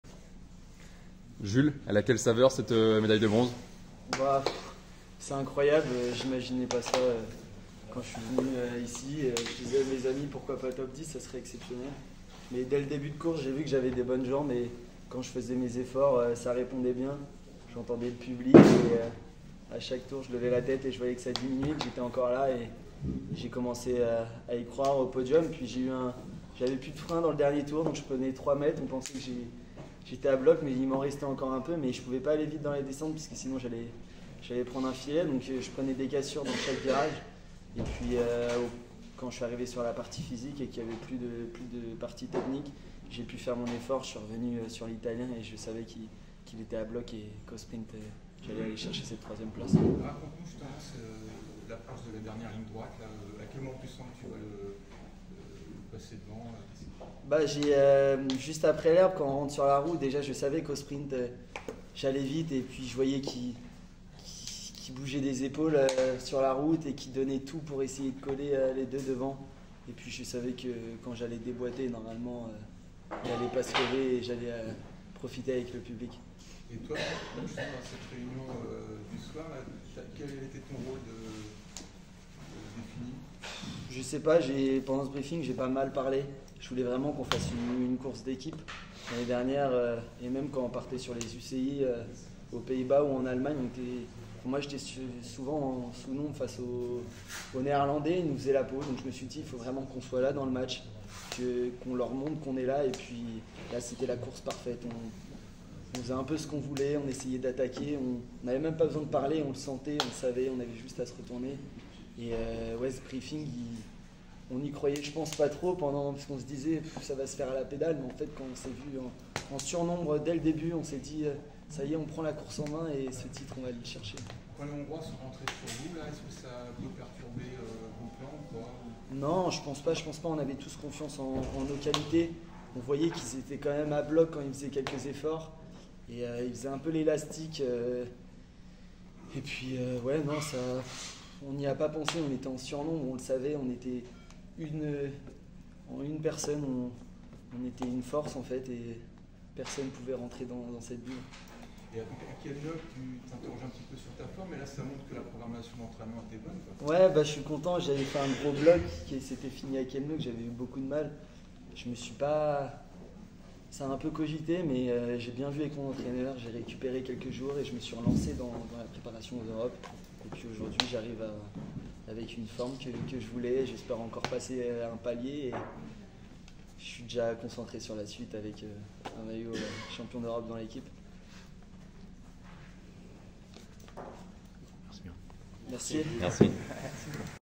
Cyclo-cross - Championnats d'Europe - Pontchâteau 2023 - Jules Simon en Bronze : "C'est incroyable"
Jour de fête pour l'équipe de France à Pontchâteau ! Les coureurs tricolores sont en feu sur ces Championnats d'Europe de cyclo-cross et apportent déjà à la France un troisième titre européen... en seulement trois courses ! Après le relais mixte vendredi et Célia Géry quelques minutes plus tôt, c'est Aubin Sparfel qui s'est paré d'or ce dimanche matin sur la course Juniors. Le pensionnaire d'AG2R Citroën U19 Team a remporté son duel au sprint avec le Hongrois Zsombor Takacs après plus de 43 minutes d'efforts. Un bonheur n'arrivant jamais seul, Jules Simon accompagne son compatriote sur le podium et ajoute du bronze à la razzia française. Paul Seixas (6e), Louis Tanguy (8e) et Maxime Vezie (9e) participent également à la fête. Aubin Sparfel succède au palmarès à son aîné Léo Bisiaux, qui participera à l'épreuve Espoirs ce midi.